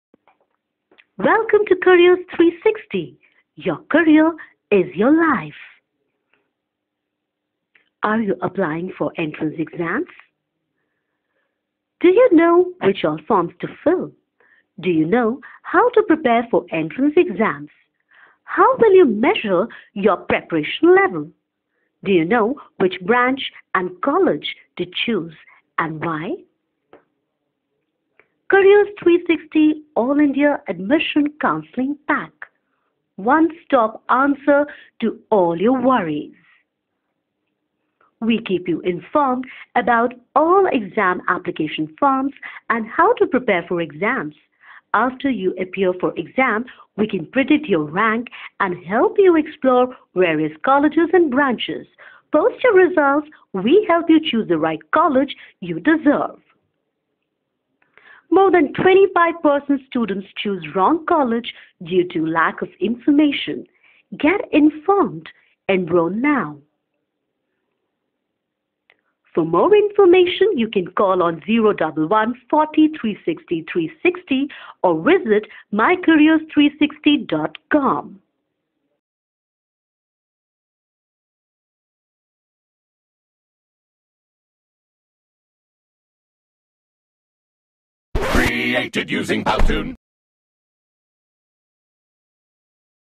Careers360-All India Engineering Admission Counselling Pack-Introduction
Are you aspiring to get admission into one of the best engineering colleges of India?

All India Engineering Admission Counselling is an end-to-end guidance package designed by the top experts in domain of educational counselling to answer all your queries. It addresses all what has been worrying you for the admission process.

or call on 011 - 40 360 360.